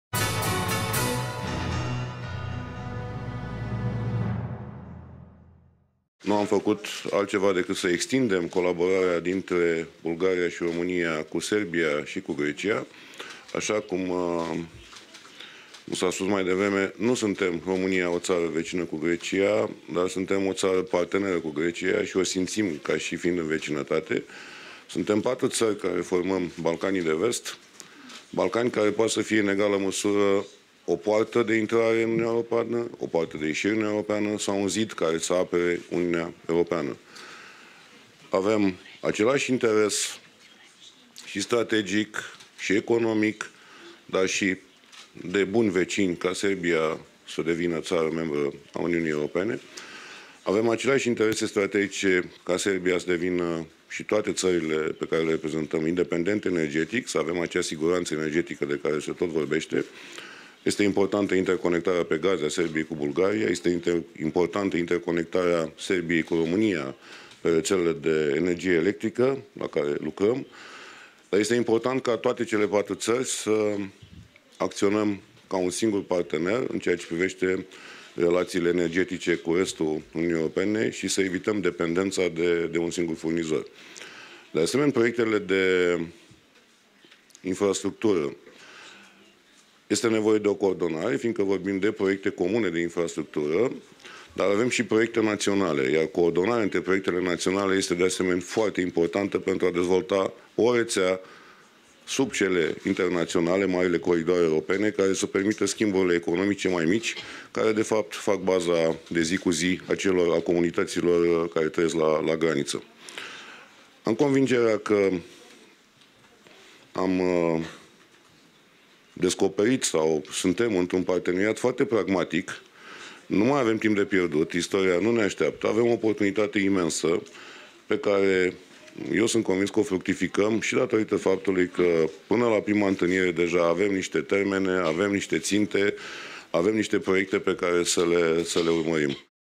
Tudose: Suntem patru țări care formăm Balcanii de Vest, un zid care să apere UE
"Nu am făcut altceva decât să extindem colaborarea dintre Bulgaria și România cu Serbia și cu Grecia. (...) untem patru țări care formăm Balcanii de Vest, Balcani care pot să fie în egală măsură o poartă de intrare în Uniunea Europeană, o poartă de ieșire din Uniunea Europeană sau un zid care să apere Uniunea Europeană", a declarat premierul României, Mihai Tudose, într-o conferință de presă comună cu președintele sârb, Aleksandar Vucic, și cu premierii bulgar și grec, Boiko Borisov și Alexis Tsipras.